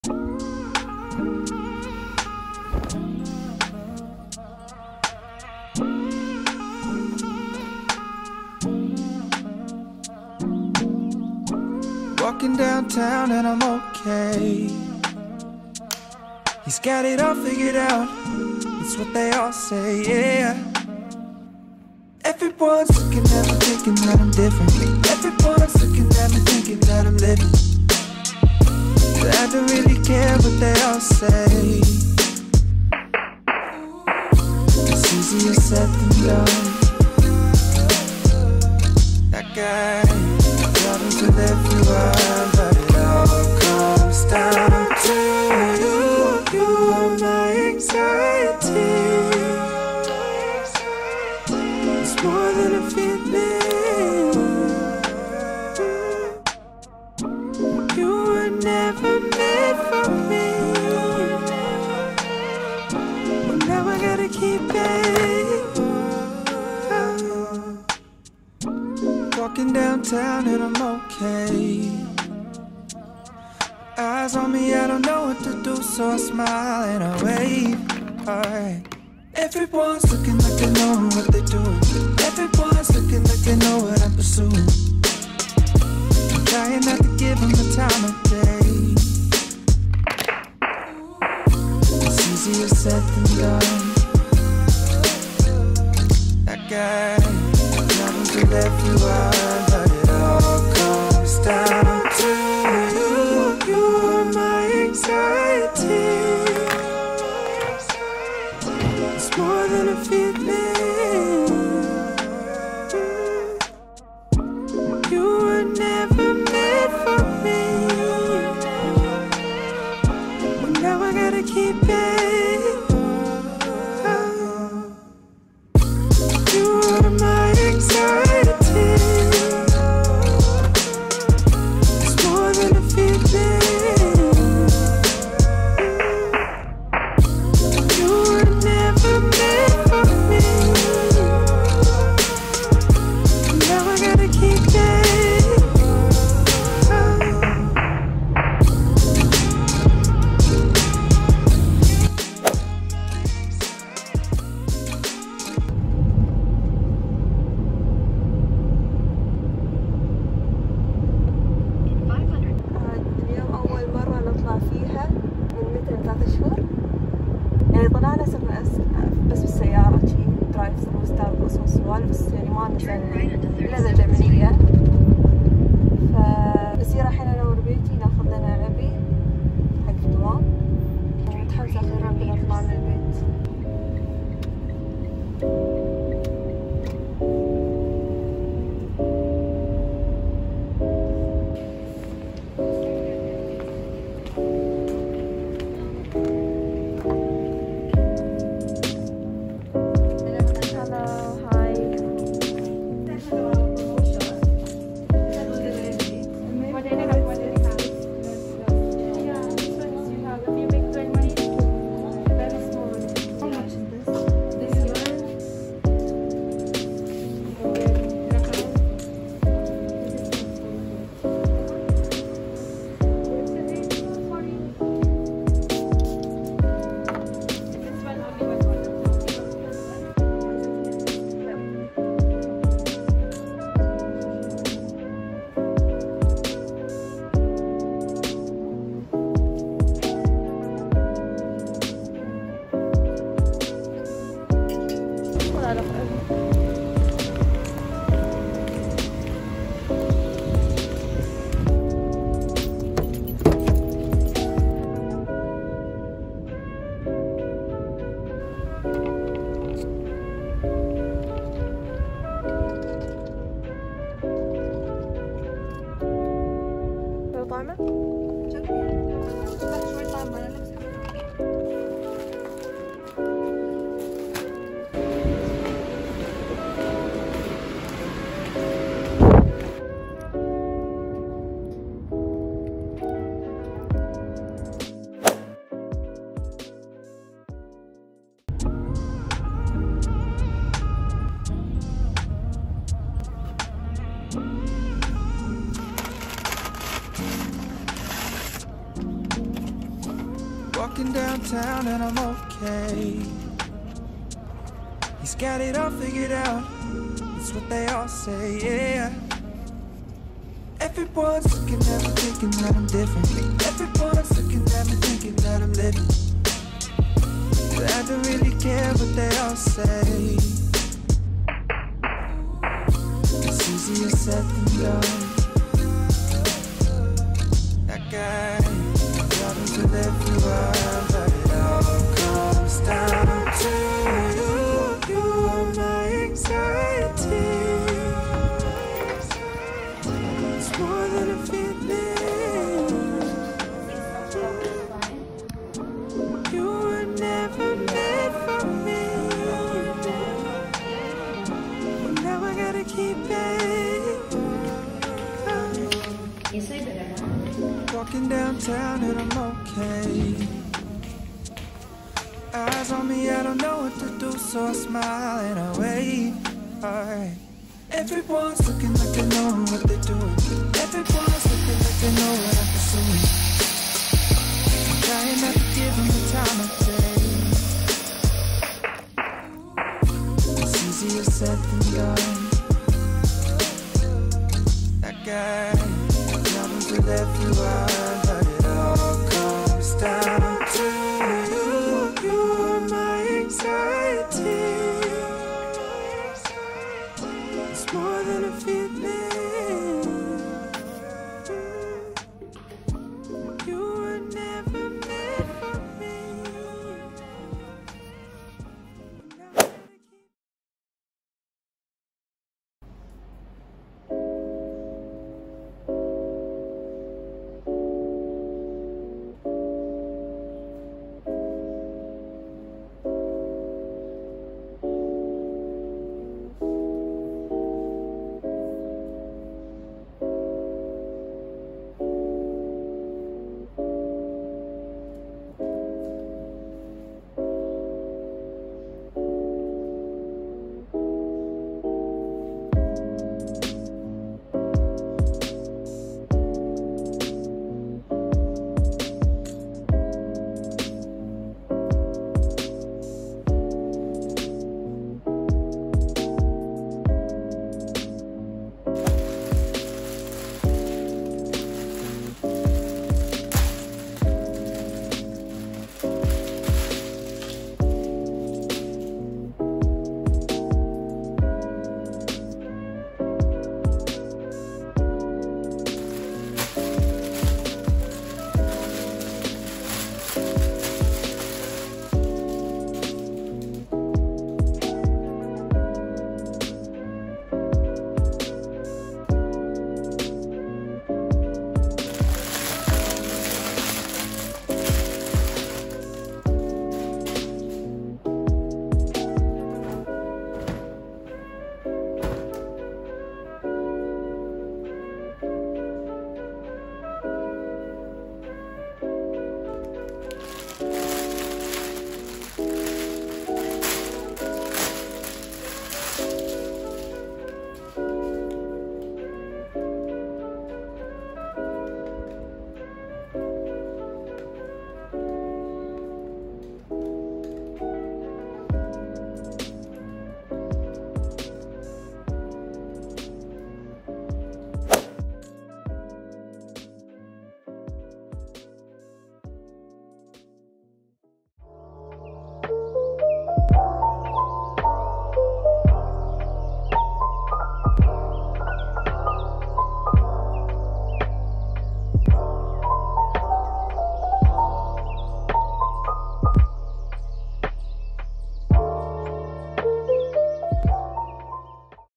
Day in the life: escaping quarantine after 3 months: first day at work + abaya shopping + nail salon
Day in the life: adulting series 101, going to work for the first time ever!! I also did some abaya shopping for work, and went to the nail salon for the first time in months!!